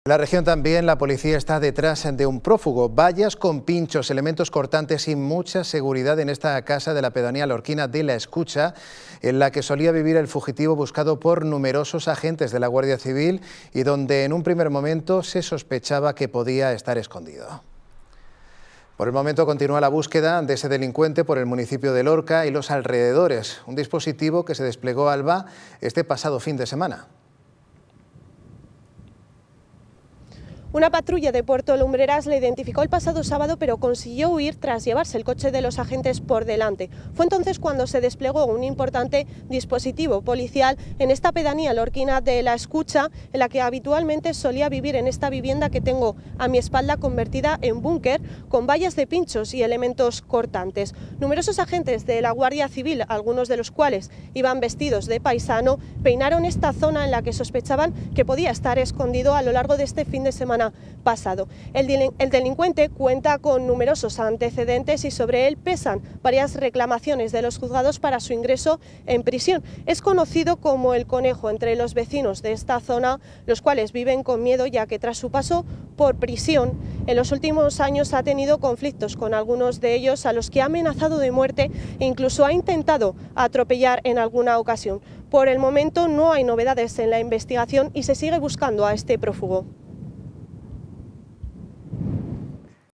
Siguen buscando a un prófugo en Lorca y alrededores | La 7
Se trata de un hombre que fue visto y reconocido por una patrulla de la Guardia Civil de Puerto Lumbreras durante el sábado pasado, pero posteriormente se dio a la fuga. En ese momento, la Guardia Civil inició una búsqueda organizada por toda la zona de pedanía de La Escucha, donde vivía, y otros puntos del municipio lorquino. El delincuente vivía en una casa convertida en un 'búnker'.